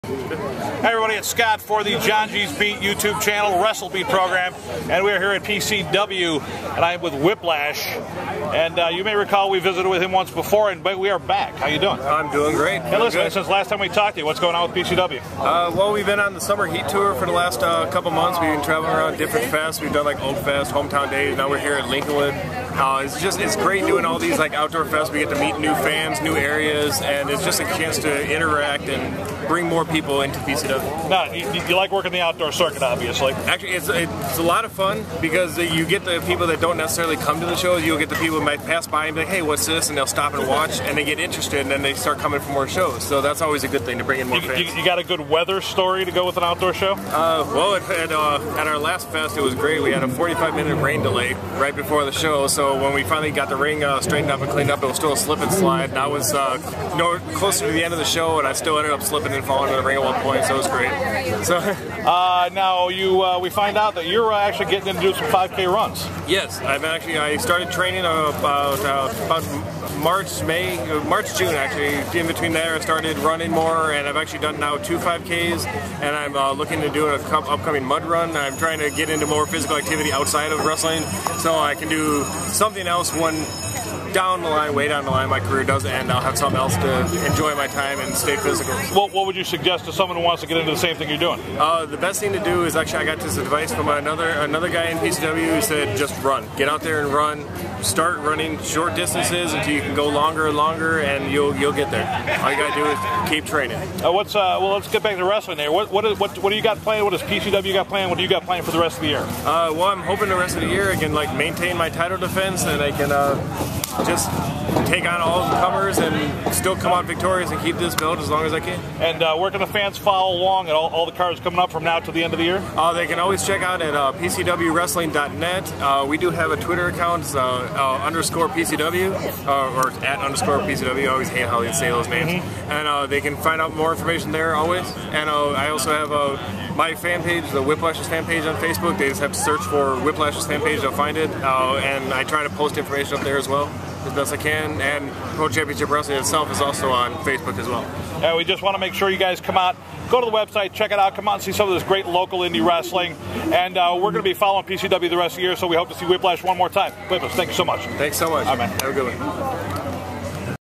On the Beat with Whiplash of Pro Championship Wrestling (PCW)
At Lincolnwood Fest on 8/5/12, we enjoyed a great afternoon of PCW (Pro Championship Wrestling).  We also got several interviews.  Look for them here on the JohngysBeat YouTube channel.
In this interview, we talked to Whiplash, PCW Midwest champ.  In this second interview with Whiplash, we talked about wrestling outdoors, his interests outside of the ring and more.
For more wrestling interviews, check out the JohngysBeat.com wrestling playlist here on YouTube.  Also check out for interviews, pictures, stories and other pop culture fun.